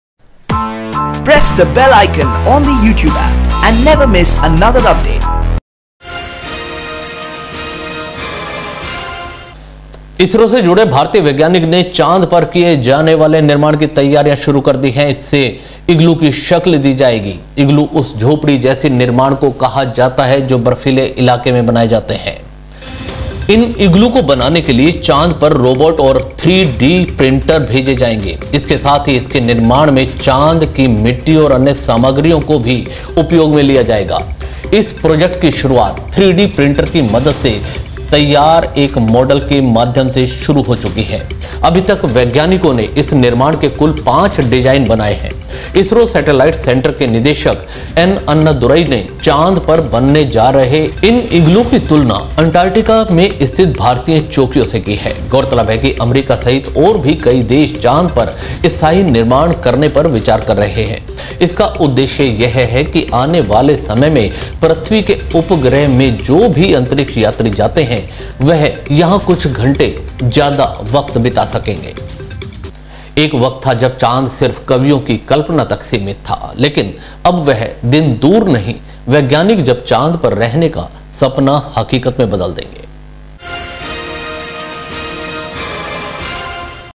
चांद पर आशियाना बनाएंगे वैज्ञानिक, इसरो ने काम किया आरंभ
इसरो से जुड़े भारतीय वैज्ञानिकों ने चांद पर किए जाने वाले निर्माण की तैयारियां शुरू कर दी है। इसे इग्लू की शक्ल दी जाएगी। इग्लू उस झोपड़ी जैसे निर्माण को कहा जाता है जो बर्फीले इलाके में बनाए जाते है।
इन इग्लू को बनाने के लिए चांद पर रोबोट और 3डी प्रिंटर भेजे जाएंगे। इसके साथ ही इनके निर्माण में चांद की मिट्टी और अन्य सामग्रियों को भी उपयोग में लाया जाएगा। इस प्रोजेक्ट की शुरूआत 3डी प्रिंटर की मदद से तैयार एक मॉडल के माध्यम से शुरू हो चुकी है। अभी तक वैज्ञानिकों ने इस निर्माण के कुल पांच डिजाइन बनाए है। इसरो सैटेलाइट सेंटर के निदेशक एन. अन्नादुराई ने चांद पर बनने जा रहे इन इग्लुओं की तुलना अंटार्टटिका में स्थित भारतीय चौकियों से की है। गौरतलब है कि अमेरिका सहित और भी कई देश चांद पर स्थायी निर्माण करने पर विचार कर रहे हैं। इसका उद्देश्य यह है कि आने वाले समय में पृथ्वी के उपग्रह में जो भी अंतरिक्ष यात्री जाते हैं, वह यहां कुछ घंटे ज्य़ादा वक्त बिता सकें।
एक वक्त था जब चांद सिर्फ कवियों की कल्पना तक सीमित था। लेकिन अब वह दिन दूर नहीं, वैज्ञानिक जब चांद पर रहने का सपना हकीकत में बदल देंगे।